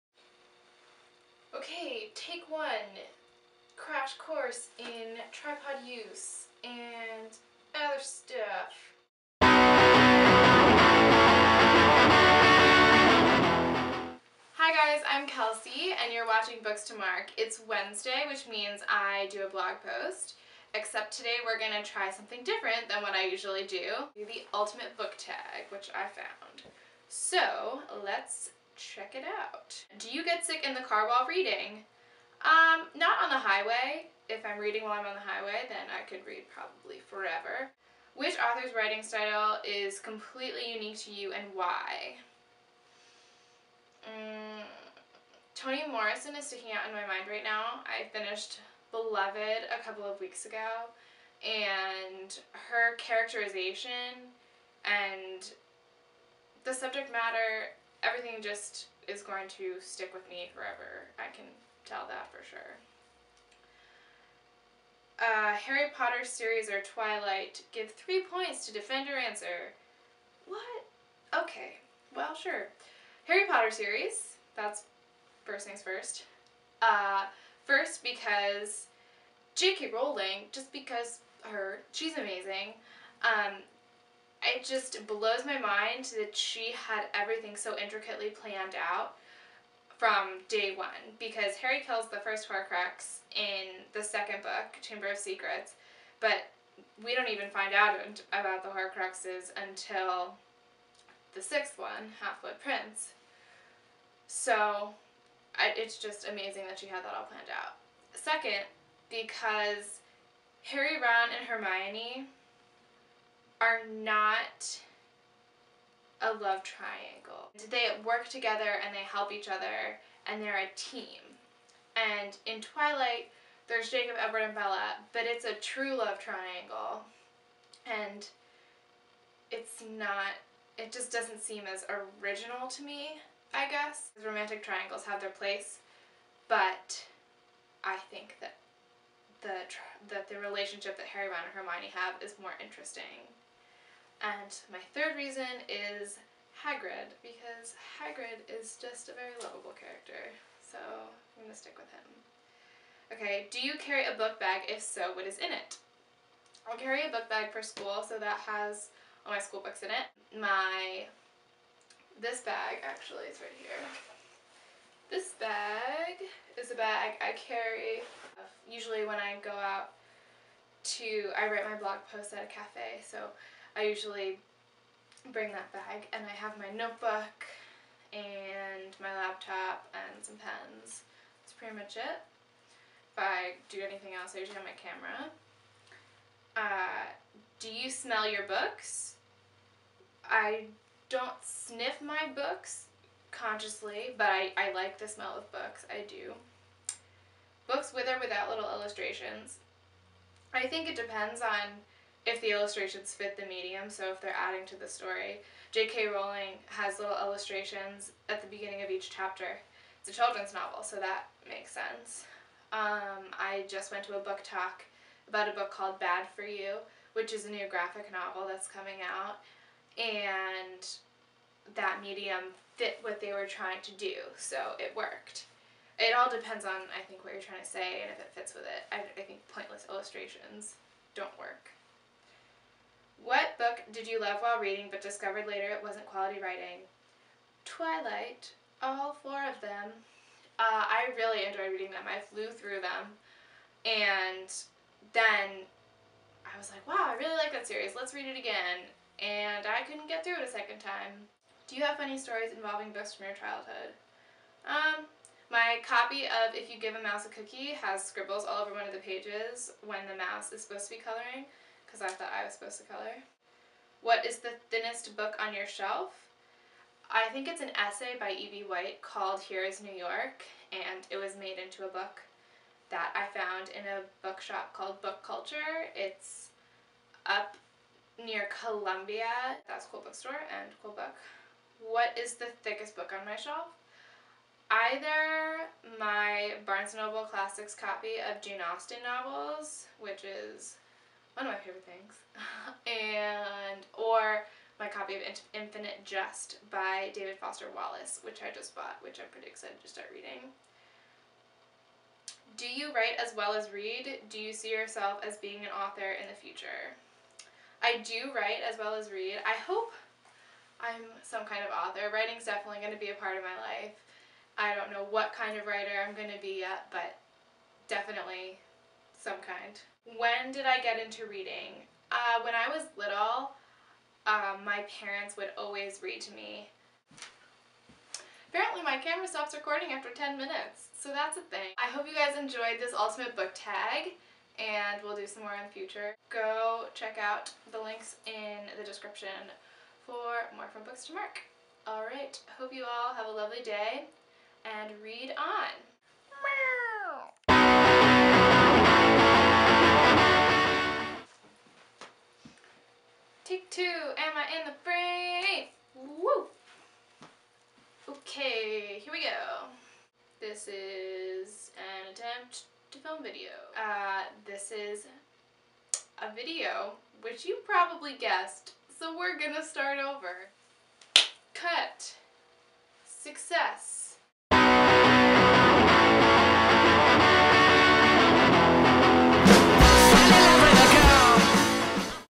Ultimate Book Tag
In which I answer some questions about my reading life. Let me know what you think in comments and pressing the thumbs up button!